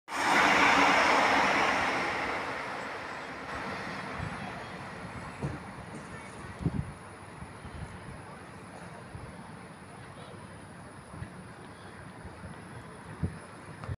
NORTHERN.150271 PASSING PARKGATE ROTHERHAM.25 2 23.NEIL HAYTON RAILWAY MEMORIES.
2R37 14.24 ADWICK TO SHEFFIELD SERVICE.